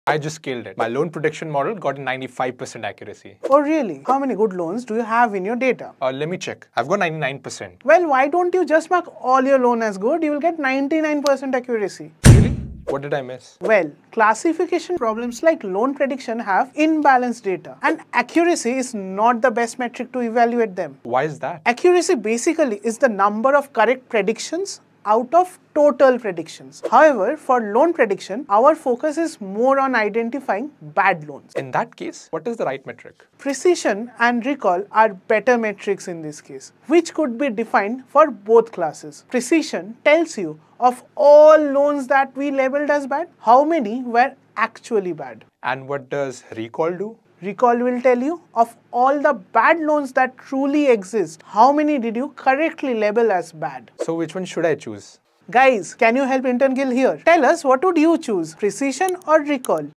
Precision & Recall - Intern Gill needs help!
Accuracy is not always the best metric to evaluate imbalanced data. ❎ ❎
- Especially in classification models like Loan Prediction.  💵 💵
- Accuracy basically is the number of correct predictions out of total predictions.🔍 🔎
- In these cases, Using accuracy here is like using a telescope to examine microorganisms.🔭.🔭

What do you think is the right parameter?❓❓
- Precision and Recall are better metrics to evaluate both classes in classification models. ✅ ✅
- Precision is of all the loans that we labeled as 'Bad', how many were actually 'Bad'? 🔬🔬
- Recall is of all the 'Bad Loans' that truly exist, how many did you correctly label as 'Bad'. 📏📏

🔥🔥Sometimes selection of accurate metrics to see how your model works perfectly is extremely important!💬 ✅